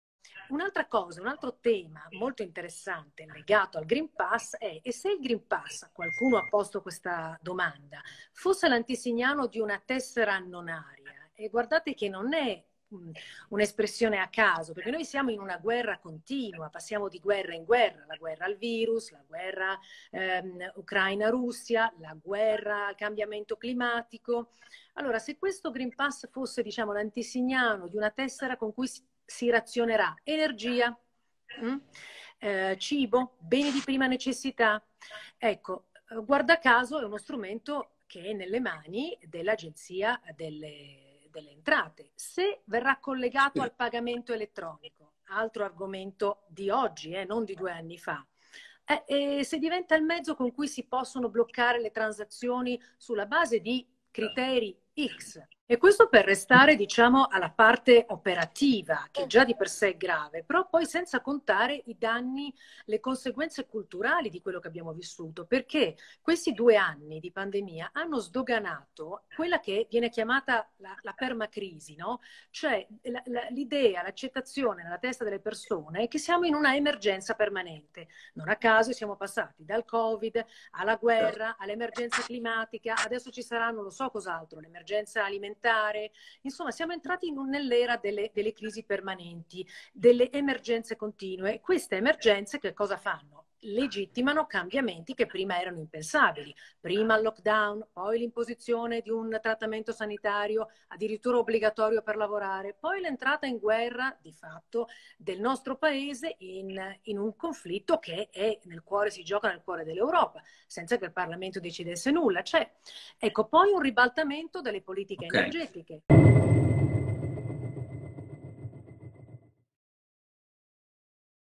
GREEN PASS COME TESSERA DI RAZIONAMENTO DEL FUTURO
Se il greenpass fosse l'antesignano di una tessera annonaria per razionare energia/cibo?
Già adesso curiosamente gestioto dalla Agenzia delle Entrate, se collegato al pagamento elettronico può diventare mezzo per bloccare i cittadini sulla base di decisioni x.

Ma il Covid ha portato anche allo sdoganamento di una serie di cose: l’avvio della ""permacrisi"", cioè l'emergenza permanente (dal Covid siamo passati alla guerra, alla crisi energetica); la legittimazione, in nome di questa emergenza, di cambiamenti impensabili: lockdown, imposizione di trattamento sanitario posto perfino come obbligo per lavorare, partecipazione dell’Italia a una guerra nel cuore dell’Europa (senza che il parlamento si sia mai espresso), ora un ribaltamento delle politiche energetiche per raggiungere gli obiettivi climatici...

(dalla diretta Instagram con La Verità)